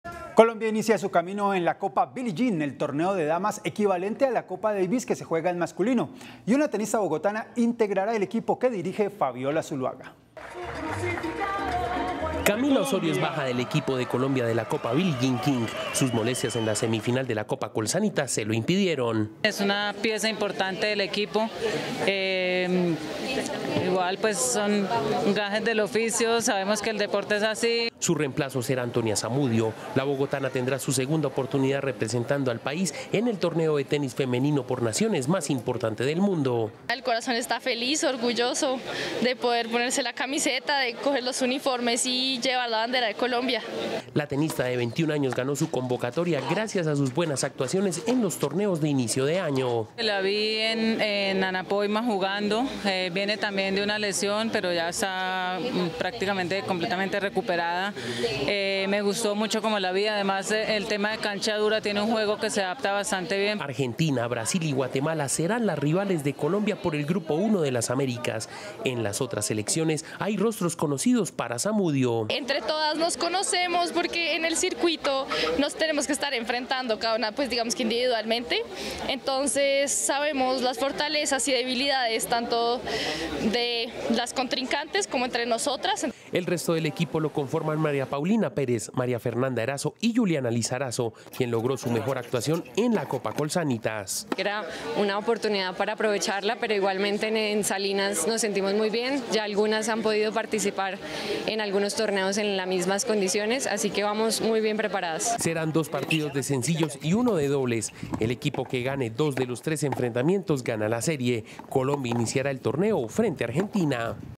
Tenista bogotana jugará con Colombia en la Copa Billie Jean King
DeportesCapital | Colombia inicia su camino en la Copa Billie Jean King. La bogotana Antonia Samudio tendrá su segunda oportunidad representando al país en el torneo de tenis femenino por naciones más importante del mundo.